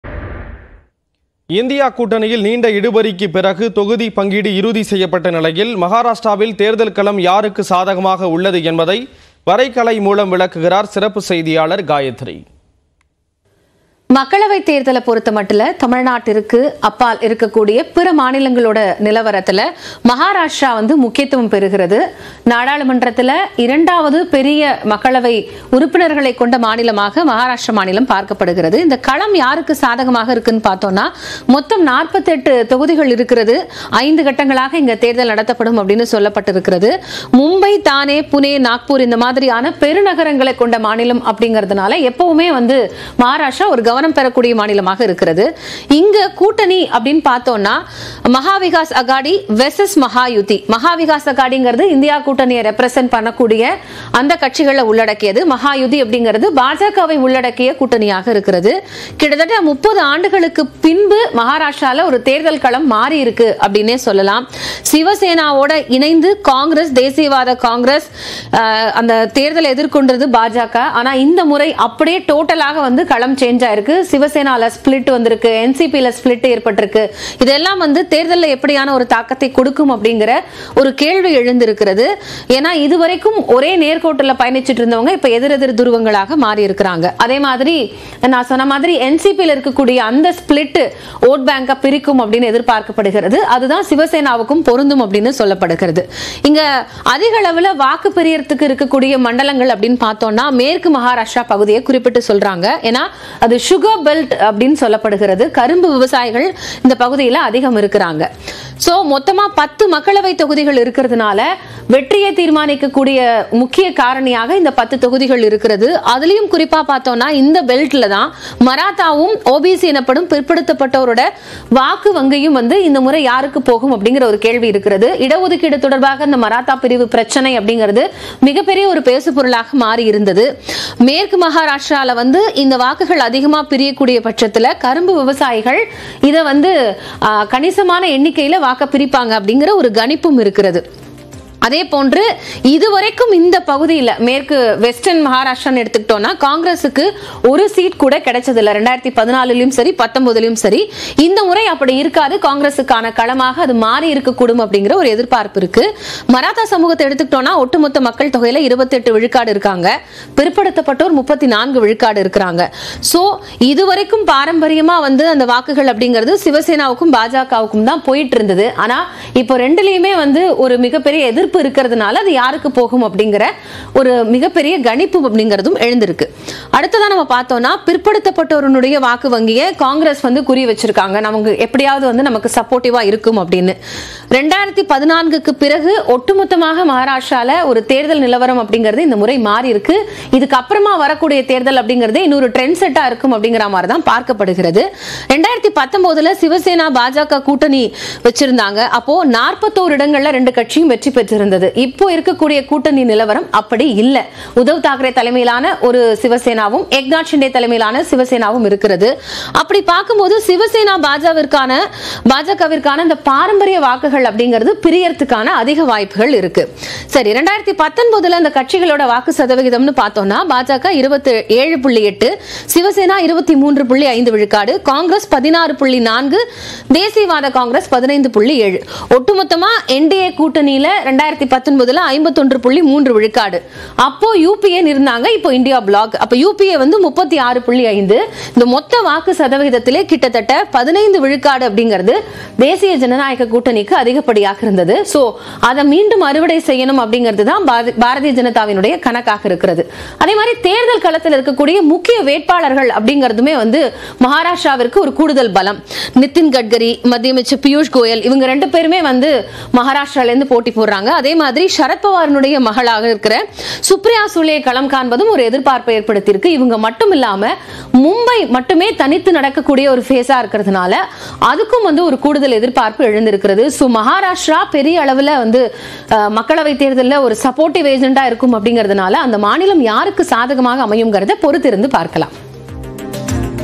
மகாராஷ்டிரா மாநிலத்துல எந்த கூட்டணி ஜெயிக்க வாய்ப்பு இருக்கு? | Maharashtra | India Alliance
list=PLZjYaGp8v2I8Jl8PfHo2MQdWKs42-15cX